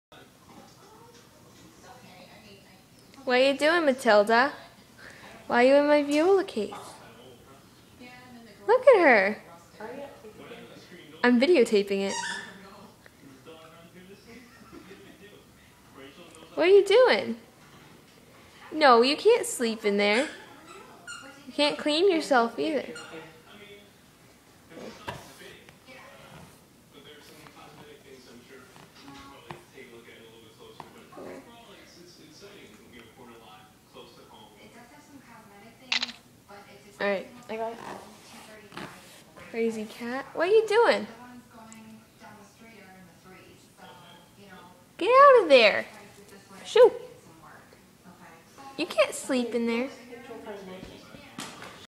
Matilda in my Viola Case....
I have the weirdest cat ever. Like seriously this is the least weird thin she's done all week. :) but that's why I -3 her!!!